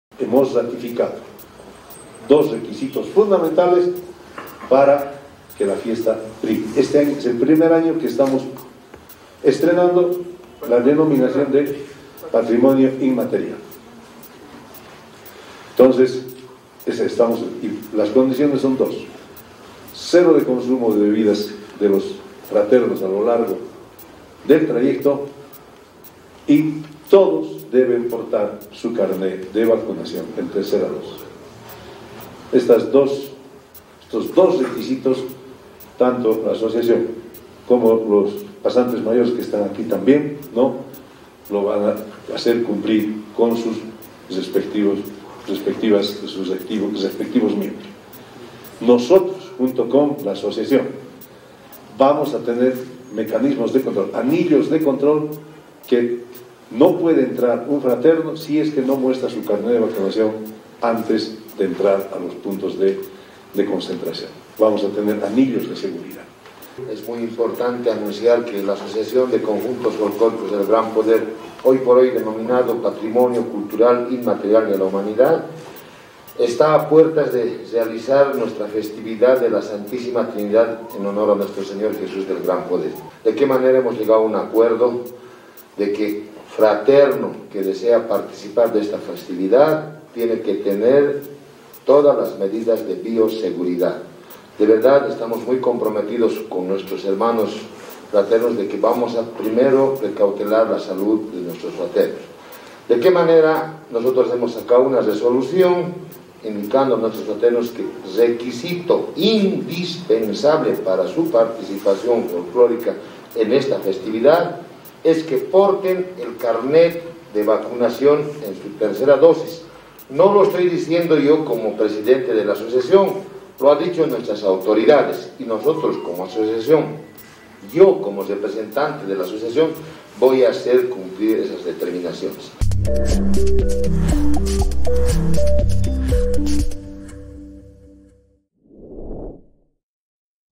Prohíben a bailarines el consumo de bebidas alcohólicas y deben presentar su carnet de vacunación
El alcalde Iván Arias junto a los dirigentes de la Asociación de Conjuntos Folklóricos del Gran Poder (ACFGP), anunciaron que durante el evento se prohibirá el consumo de bebidas alcohólicas para los bailarines y que, además, exigirán la presentación del carnet de vacunación con la tercera dosis anticovid.